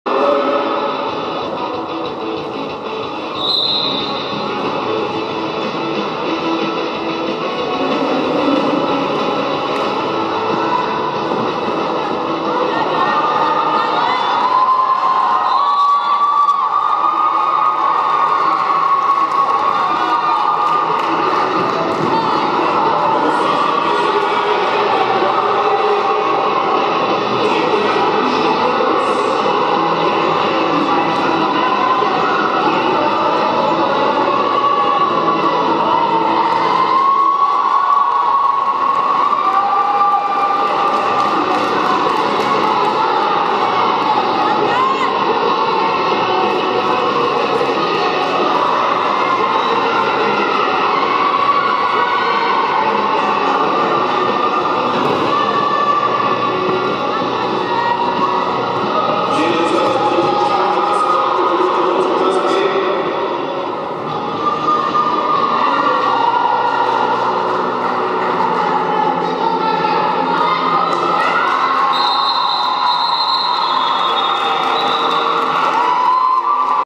Bruise Brothers: Fierce Valley Roller Girls v Dundee Roller Girls 18 Feb 2012: Final Jam
Bruise Brothers: Fierce Valley Roller Girls "Parma Violents" vs Dundee Roller Girls "Silvery Tayzers"
18th Feb 2012 @ Dundee International Sports Centre

This is the final jam of the bout.

Your jammers are: Go Go Gadget Skates (99) for Dundee
Deep Fried Pixxie (1102) for Fierce Valley.

Your announcers are Kinkhey and Fool Throttle.